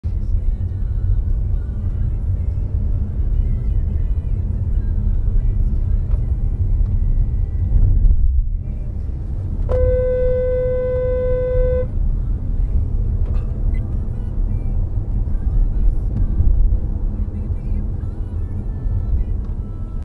2018-11-28 - grey Renault MX63XDB fails to give way at roundabout
The driver of this grey Renault MX63XDB completely fails to give way at this roundabout. The driver could not have missed seeing me coming from the right.